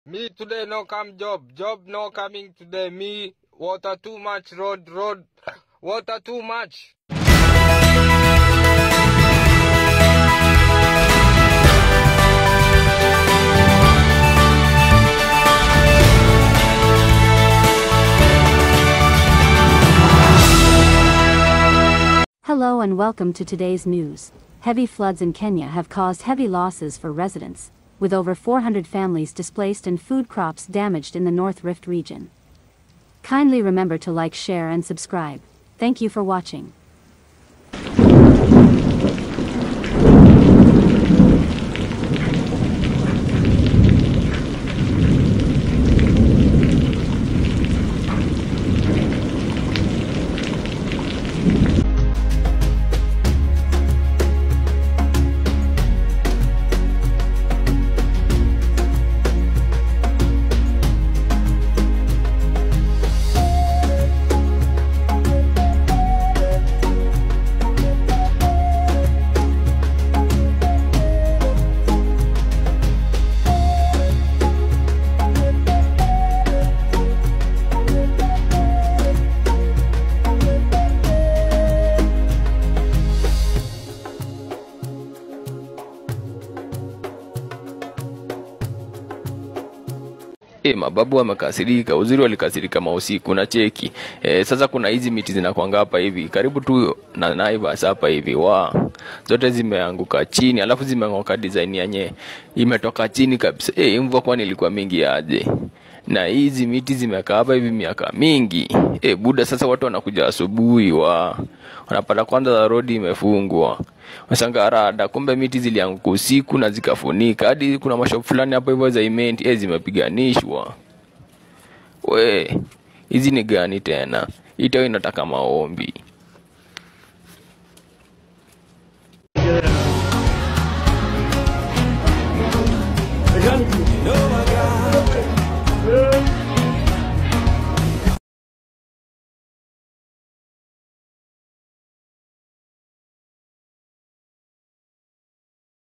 Kenya is DROWNING: Shocking Footages of the Devastating Floods
Heavy rains have unleashed a terrifying force across Kenya. What started as a downpour has become a relentless torrent, swallowing homes, separating families, and leaving a trail of heartbreaking destruction in its wake.
Click watch the video to see the unseen, feel the urgency, and discover how YOU can make a difference.